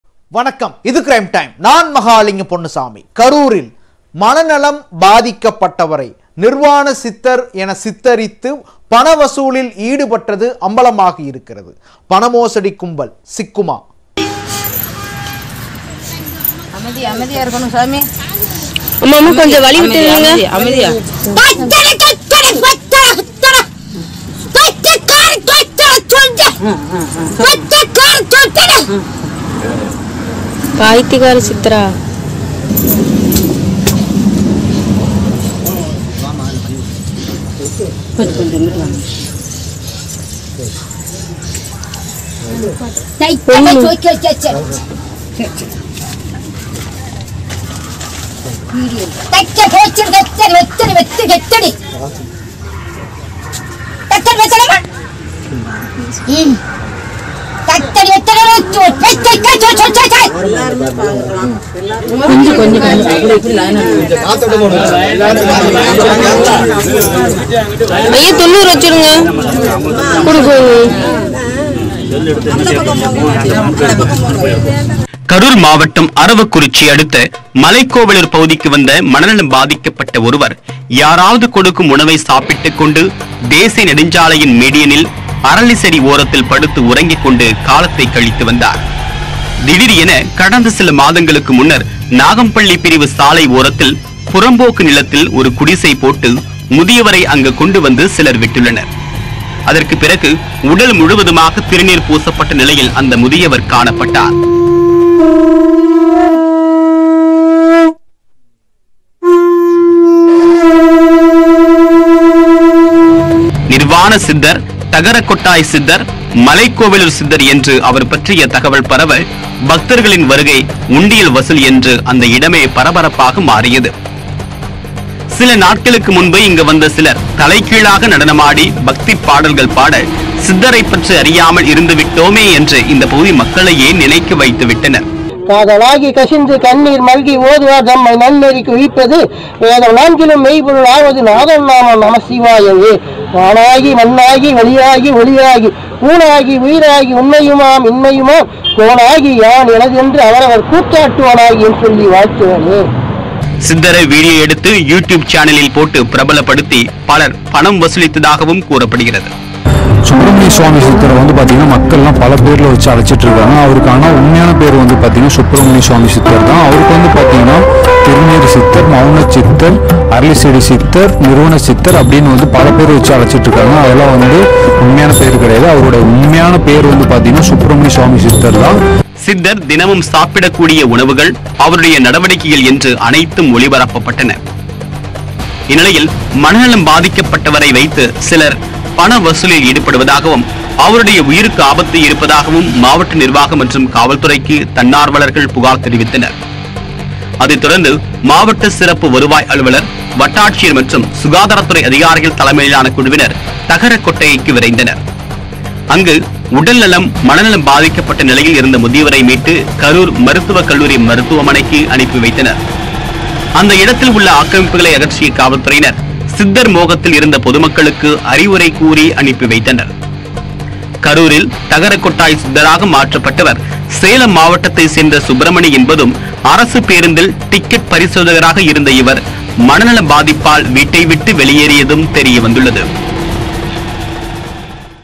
Crime Time | திடீர் சித்தரான மனநோயாளி.. பண வசூல் செய்த கும்பல்.. | Aravakurichi |Tamil News
க்ரைம் டைம் | கரூர் மாவட்டம் அரவக்குறிச்சியில் நிர்வாண சித்தர் என பிரபலப்படுத்தப்பட்ட மனநோயாளியை, மாவட்ட நிர்வாகம் மீட்டு மருத்துவமனையில் சேர்த்துள்ளது. சித்தர் பெயரில் பணம் வசூலித்த கும்பல் சிக்குமா? இது குறித்த கூடுதல் தகவல்களை தருகிறது நியூஸ்18 தமிழ்நாடு க்ரைம் டைம் | CrimeTime Aravakurichi க்ரைம்டைம் News18TamilNadu TamilNews n18oc_tamil-nadu

Sat Dec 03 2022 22:23:07